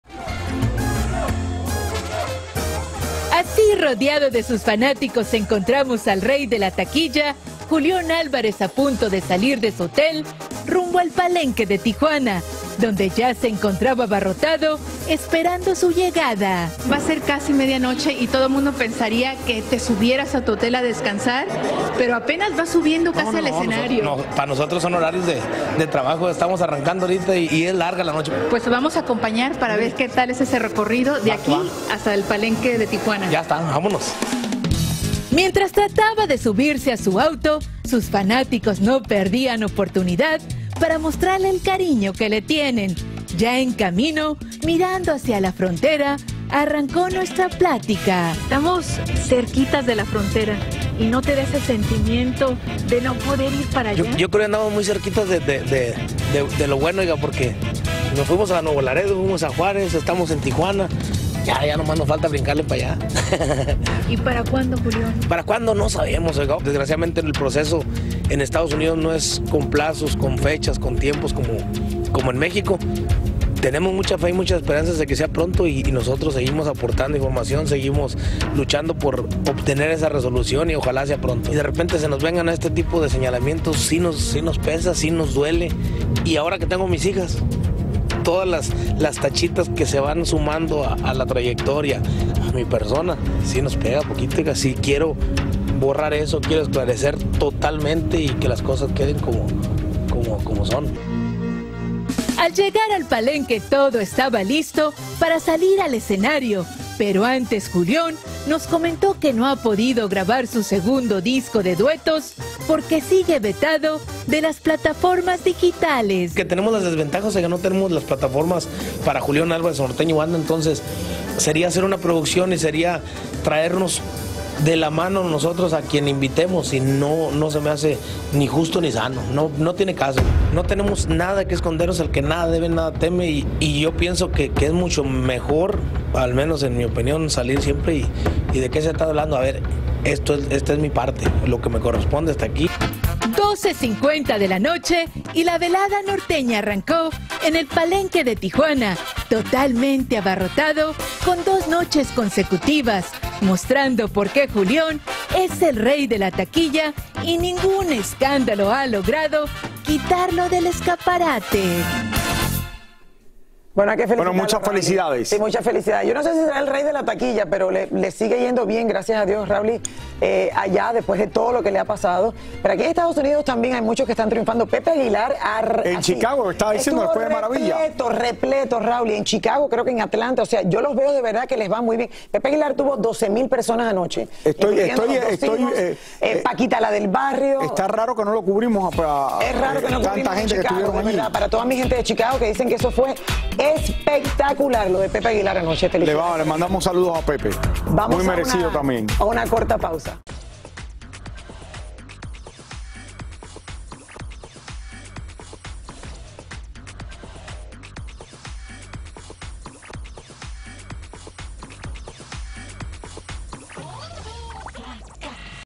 Julión Álvarez nos comparte su sentir al no poder presentarse en EEUU | GYF
El 'Rey de la Taquilla' se presentó en el palenque de Tijuana, pero en el trayecto por una de las avenidas fronterizas de México, la nostalgia se hizo presente. El cantante de regional mexicano nos compartió su sentir al estar tan cerca del país norteamericano y cómo le afecta a nivel familiar el aún no tener presentaciones en ninguna ciudad estadounidense.